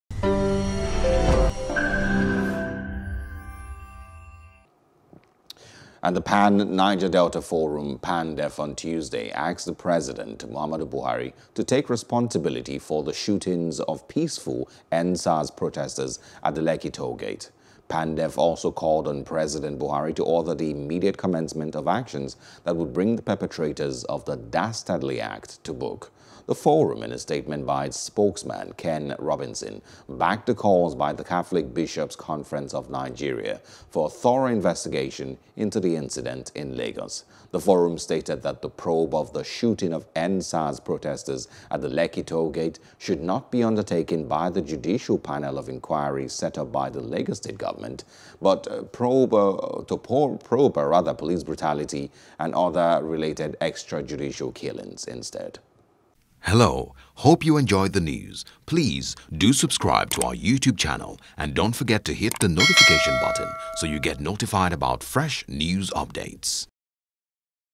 Endsars: Take Full Responsibility for Lekki Shootings PANDEF Tells Buhari (News | Nigeria)
The Pan Niger Delta Forum (PANDEF) on Tuesday asked the President, Muhammadu Buhari to take full responsibility for the shootings of peaceful EndSARS protesters at the Lekki Tollgate.